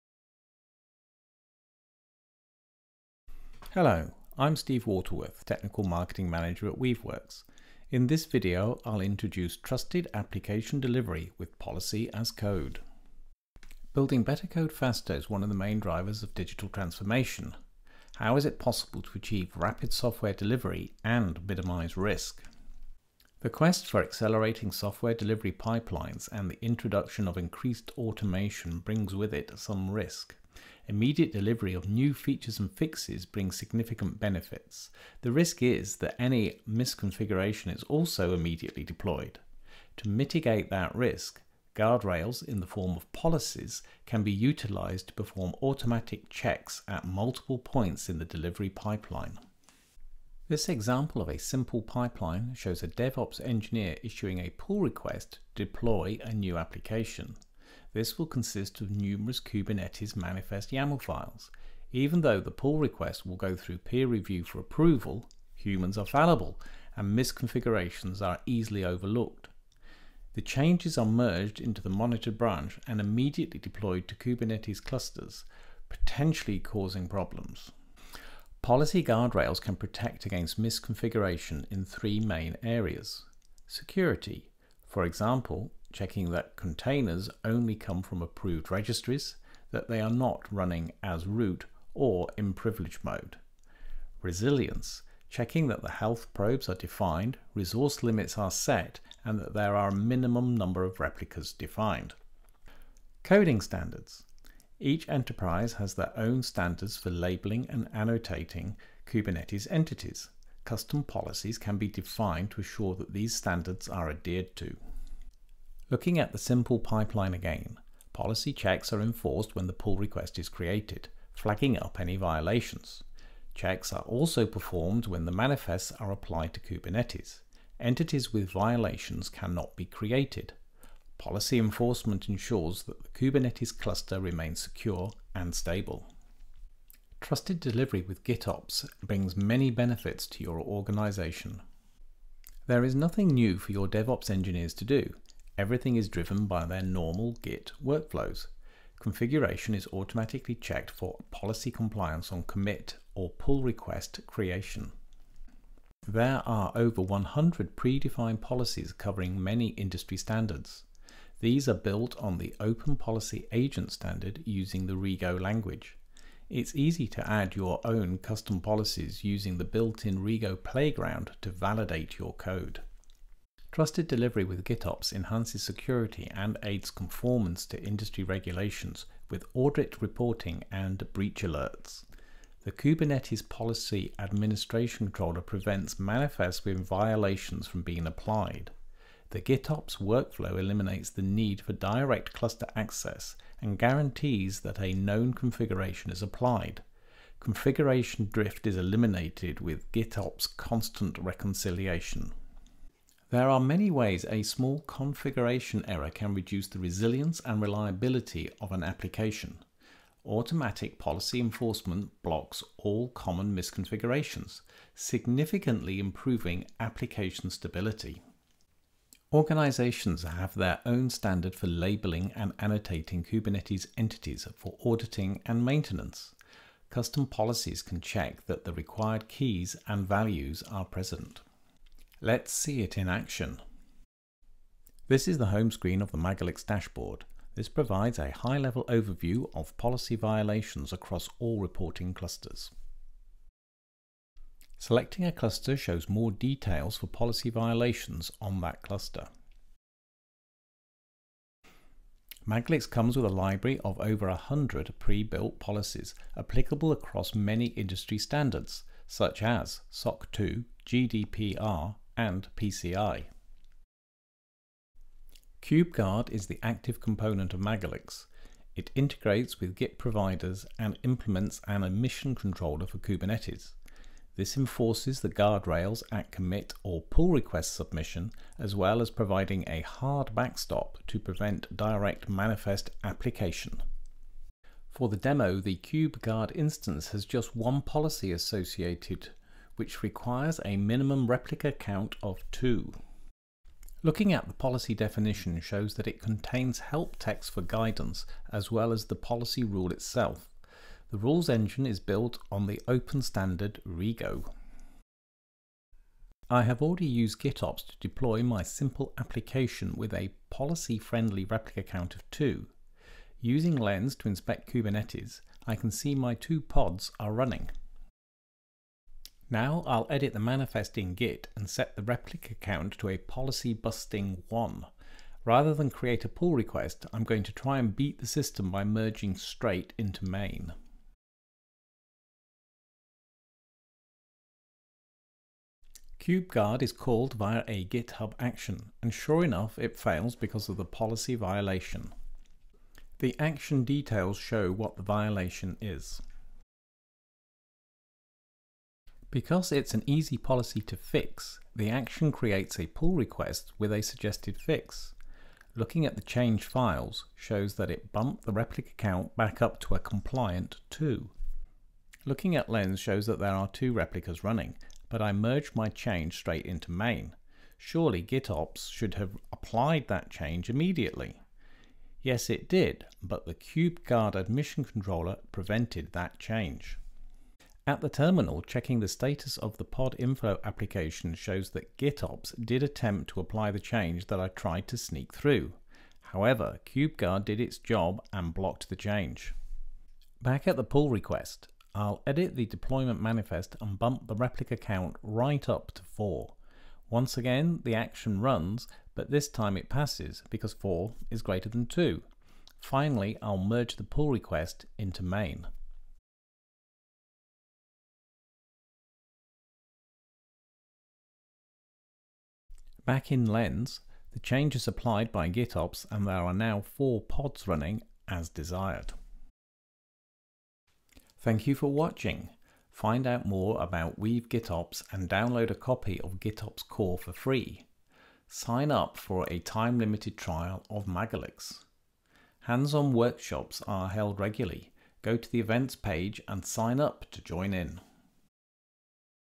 Trusted Application Delivery with GitOps and Policy as Code
Unlock the power of trusted application delivery with Policy as Code in this video presented by Steve Waterworth, Technical Marketing Manager at Weaveworks. Delve into the world of GitOps application delivery and learn how to accelerate software pipelines while minimizing risks.

Discover how Policy as Code serves as guardrails for your deployment pipelines, automating checks and preventing misconfigurations. Steve explains the three main areas where policy guardrails are crucial: security, resilience, and coding standards. With automatic policy checks at every stage of the delivery pipeline, you can ensure secure and stable Kubernetes clusters.

Explore the advantages of GitOps for trusted application delivery, where Git workflows drive configuration and over 100 predefined policies are at your disposal. Learn how to add custom policies effortlessly using the built-in Rego playground.

Steve also demonstrates the Magalix dashboard, policy violations, and the enforcement capabilities of GitOps with CubeGuard. GitOps guarantees known configurations, eliminates configuration drift, and enhances application stability.